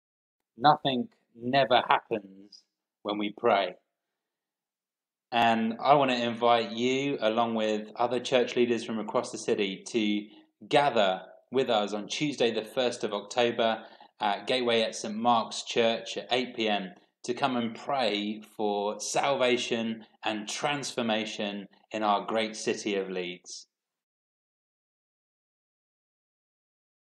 Leeds Citywide Prayer, 8pm, 1 October 2019 at Gateway at St Mark's
You are invited to Prayer for Salvation and Transformation in Leeds gathering on Tuesday 1st October, 8pm at Gateway at St Mark's, St Mark's Road, Leeds, LS2 9AF.